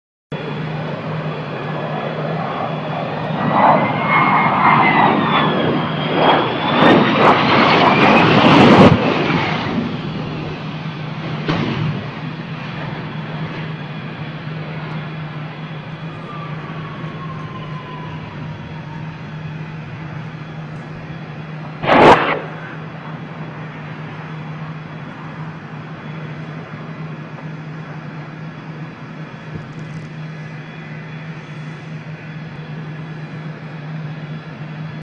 Harrier having problems
Shit happening in Kandahar, Afghanistan